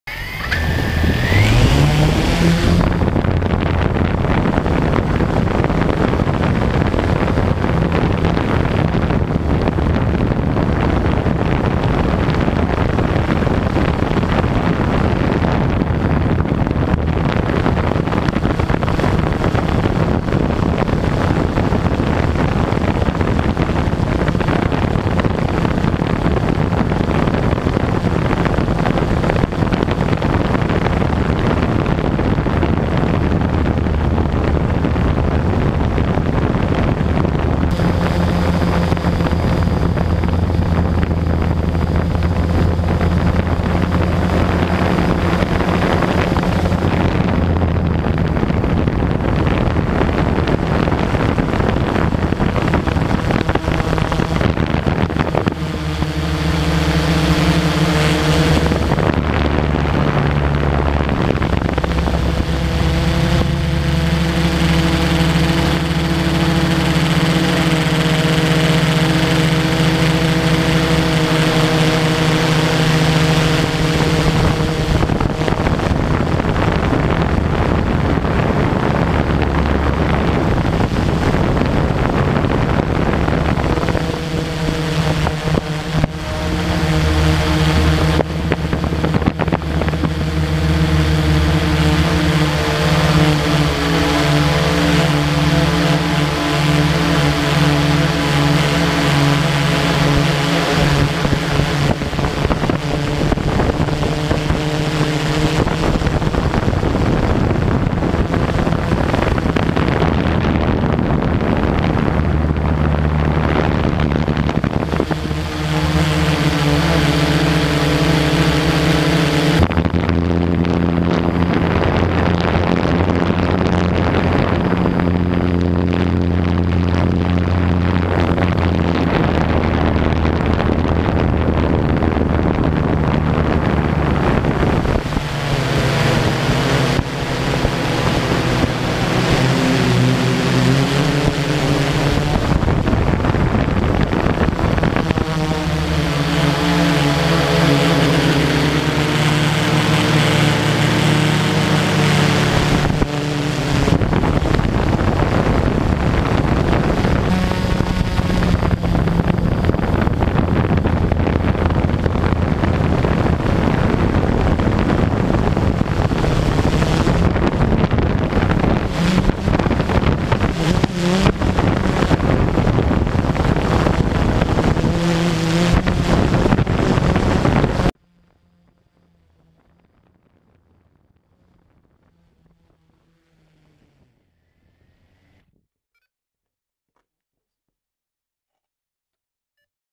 Dutch Lake Clips
Some quick unedited video of Dutch Lake Resort, Clearwater, BC.